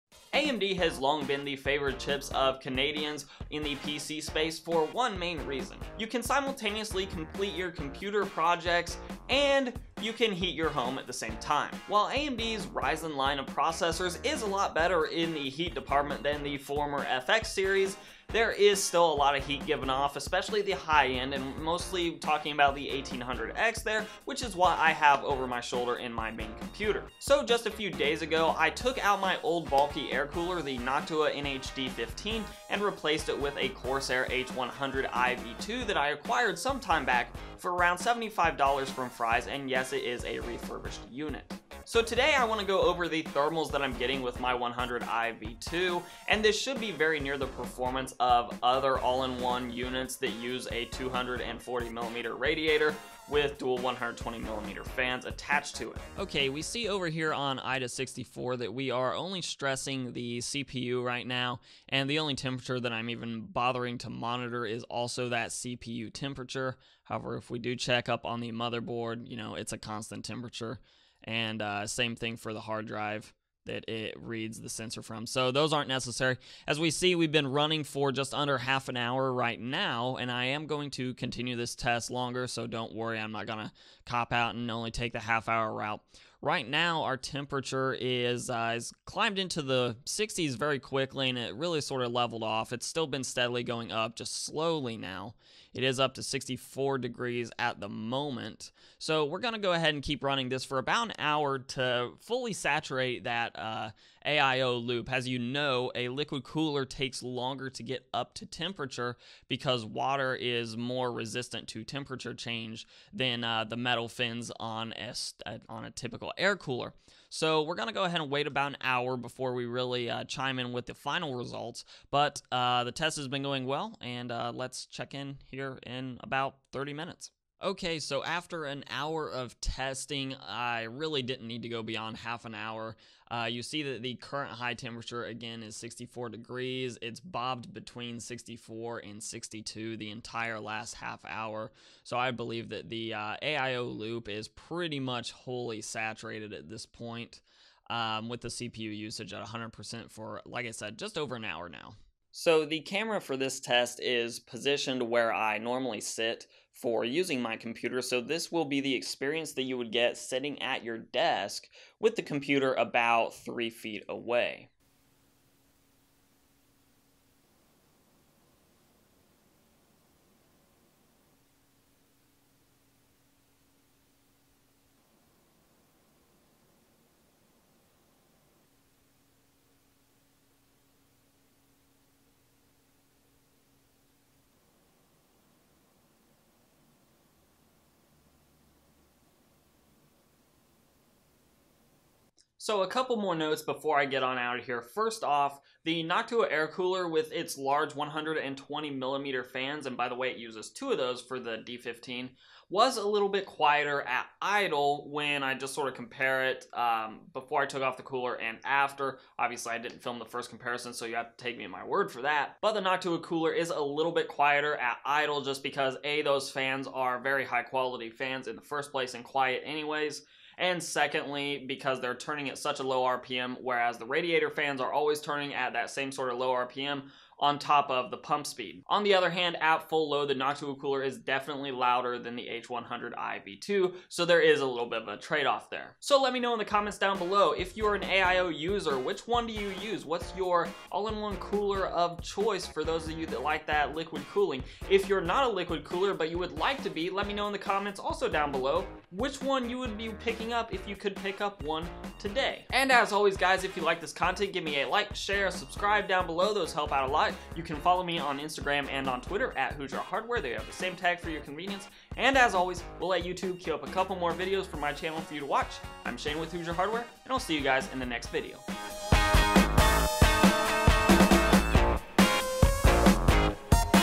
Overclocked Ryzen 1800X vs Corsair H100i V2
I recently installed my Corsair H100i V2 atop my AMD Ryzen 1800X. The processor, at the time of filming, was running at 3.94 GHz with 1.425 volts. Here are the results showing how well the liquid aio cooled the Ryzen chip.

Corsair H100i V2
Noctua NH-D15

Pair Your Cooler with a Ryzen Chip:
R5 1400
R5 1500X

R5 1600
R5 1600X

R7 1700
R7 1700X
R7 1800X